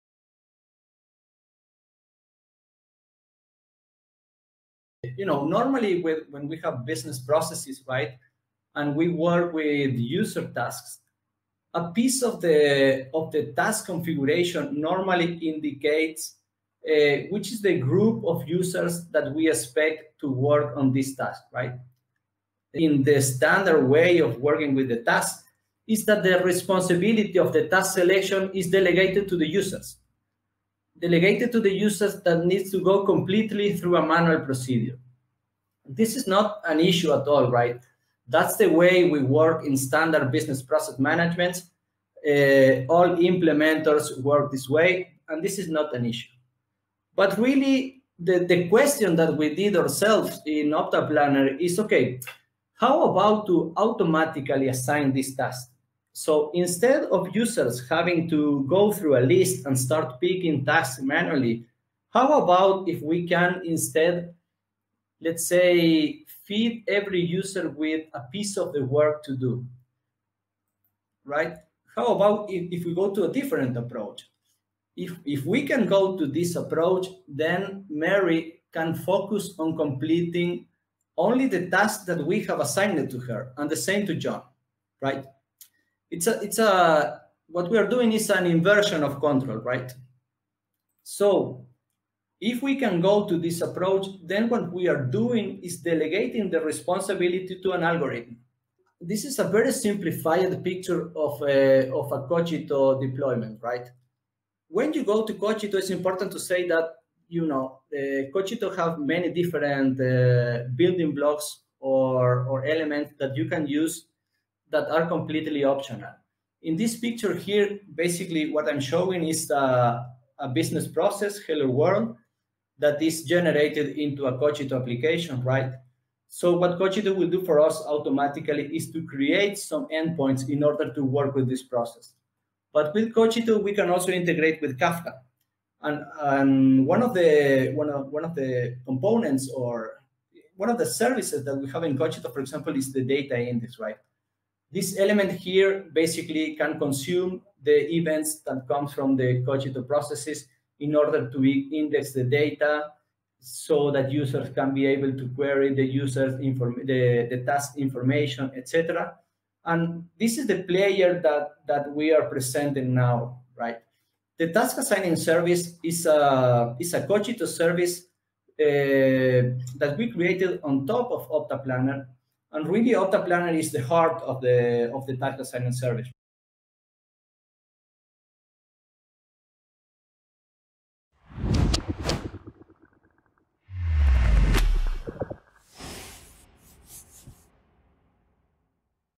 [KIE Drop] Intelligent user task assignment in business processes
It is possible today to use AI technologies to support the optimization of user task assignments in our business process flows. Instead of relying on group task assignment, you can easily rely on algorithms that can automatically assign tasks to users not only based on their current workload, but also based on skills and any other custom domain data.

This is possible through the automation of business processes using Kogito and boosted with the high performant resource planning optimization provided by OptaPlanner.

0:00 Current problem with traditional task assignment
0:46 How automatic user task assignment works
1:38 Overview of a solution implementation
2:35 The task assigning service